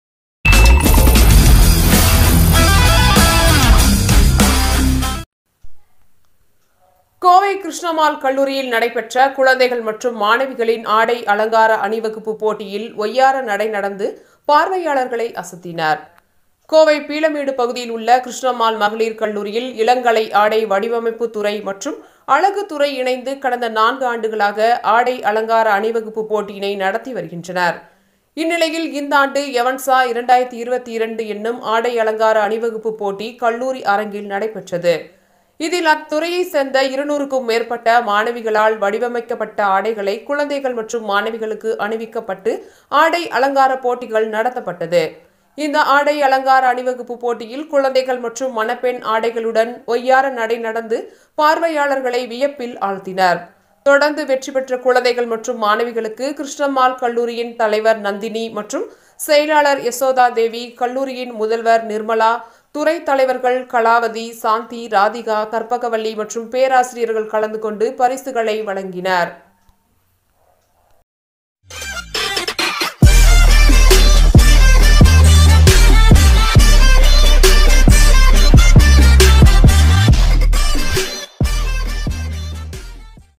கிருஷ்ணம்மாள் கல்லூரியில் குழந்தைகள் மற்றும் மாணவிகளின் ஆடை அலங்கார அணிவகுப்பு
கோவை பீளமேடு பகுதியில் உள்ள கிருஷ்ணம்மாள் மகளீர் கல்லூரியில் இளங்கலை ஆடை வடிவமைப்பு துறை மற்றும் அழுகுத் துறை இணைந்து கடந்த 4ஆண்டுகளாக ஆடை அலங்கார அணிவகுப்பு போட்டியினை நடத்தி வருகின்றனர்.இந்நிலையில் இந்த ஆண்டு எவன்சா 2022 எனும் ஆடை அலங்கார அணிவகுப்பு போட்டி கல்லூரி அரங்கில் நடைபெற்றது.இதில் அத்துறையை சேர்ந்த 200க்கும் மேற்பட்ட மாணவிகளால் வடிவமைக்கப்பட்ட ஆடைகளை குழந்தைகள் மற்றும் மாணவிகளுக்கு அணிவிக்கப்பட்டு ஆடை அலங்கார போட்டிகள் நடத்தப்பட்டது.இந்த ஆடை அலங்கார அணிவகுப்பு போட்டியில் குழந்தைகள்,மற்றும் மணப்பெண் ஆடைகளுடன் ஒய்யார நடை நடந்து பார்வையாளர்கள் வியப்பில் ஆழ்த்தினர்.தொடர்ந்து வெற்றிபெற்ற குழந்தைகள் மற்றும் மாணவிகளுக்கு கிருஷ்ணம்மாள் கல்லூரியின் தலைவர் நந்தினி மற்றும் செயலாளர் யசோதா தேவி, கல்லூரியின் முதல்வர் நிர்மலா, துறைத் தலைவர்கள் கலாவதி, சாந்தி,ராதிகா, கற்பகவல்லி மற்றும் பேராசிரியர்கள் கலந்து கொண்டு பரிசுகளை வழங்கினர்.